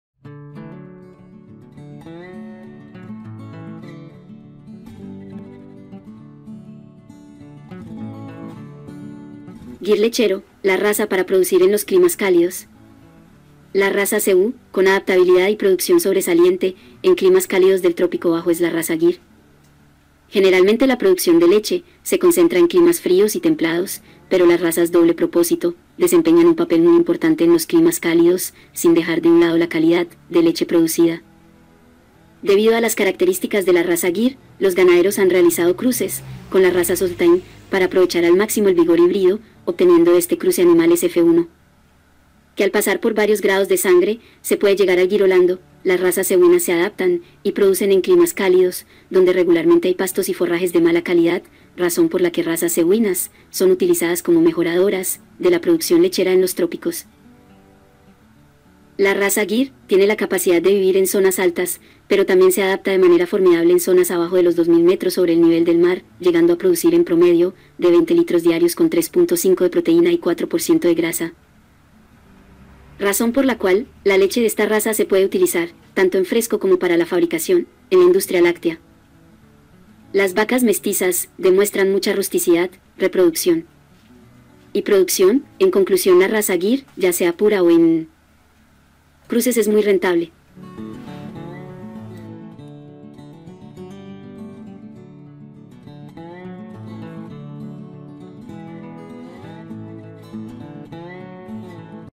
🔴 VACAS LECHERAS GYR RAZA DOBLE PROPOSITO ✅ Vacas Lecheras
Los bovinos son una especie de mamíferos usados por el hombre no solo por su carne, sino también por la leche que producen. Pues son animales que aportan muchos nutrientes y proteína a nuestra dieta. Estás son las vacas lecheras más populares en el mundo.